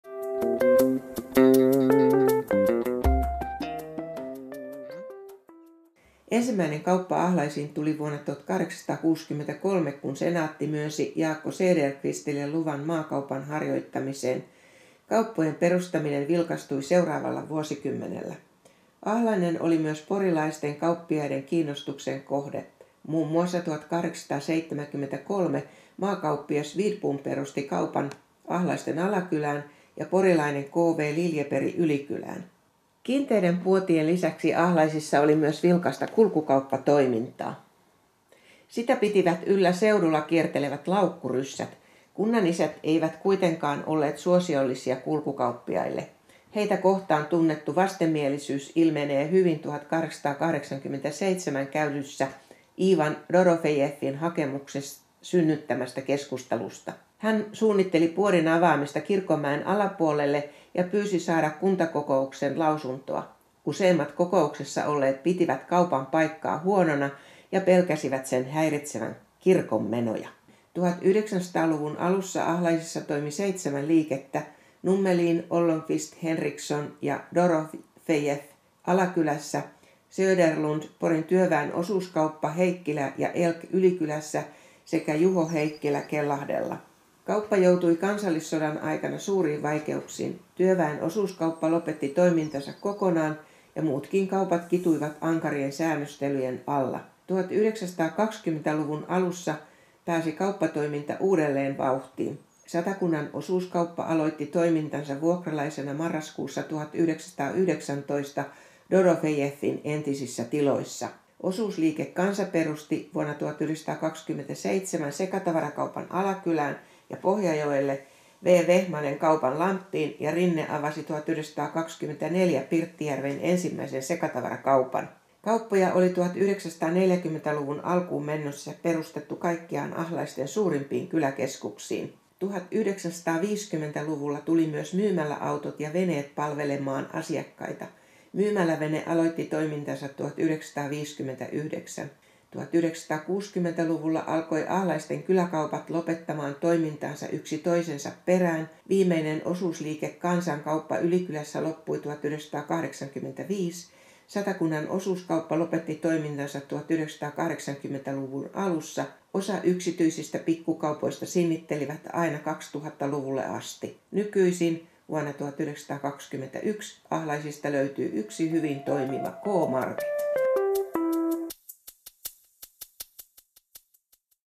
Ahlaisten kaupat pähkinänkuoressa
Kauppatarinat jatkuvat, tässä kooste kauppojen vaiheista eri vuosikymmeniltä.